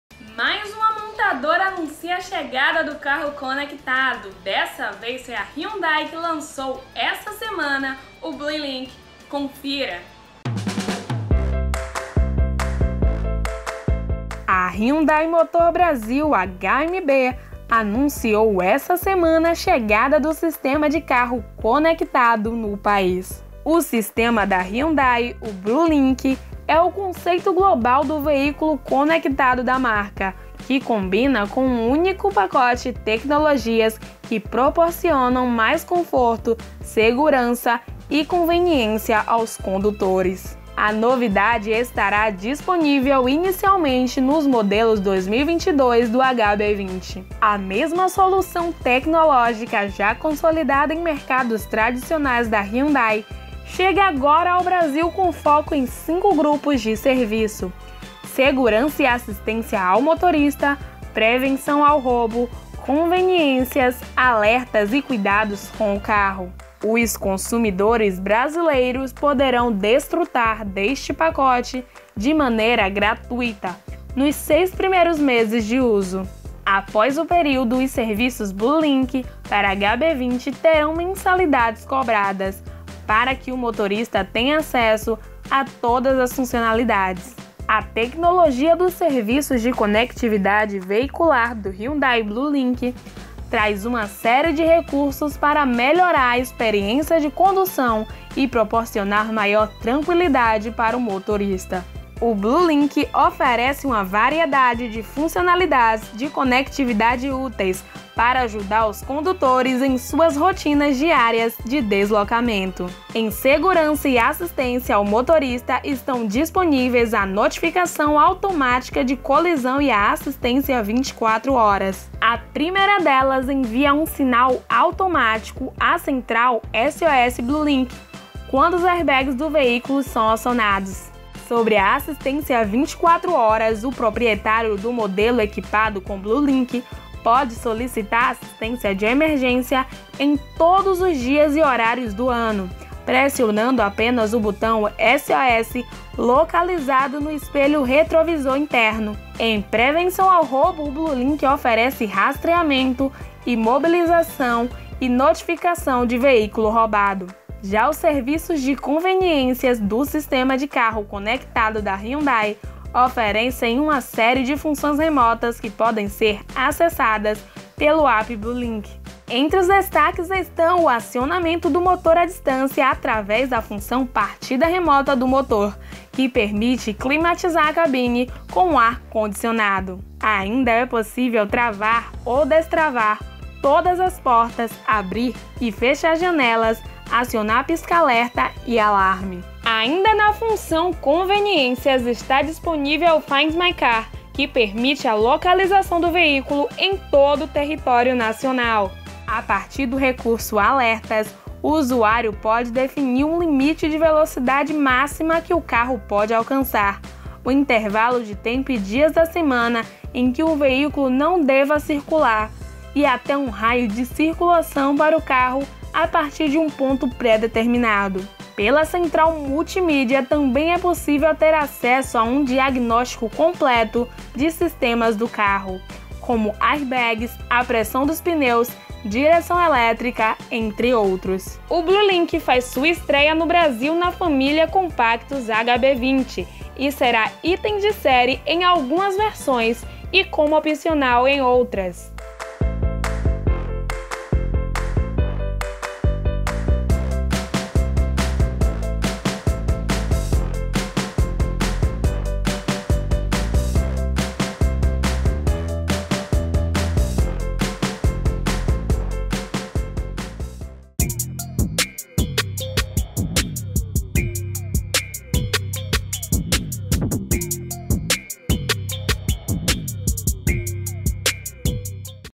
Hyundai Bluelink - Carros conectados - O que é e para que serve?
Mais uma montadora anuncia a chegada do carro conectado. Desta vez foi a Hyundai que lançou, essa semana, o Bluelink. Confira.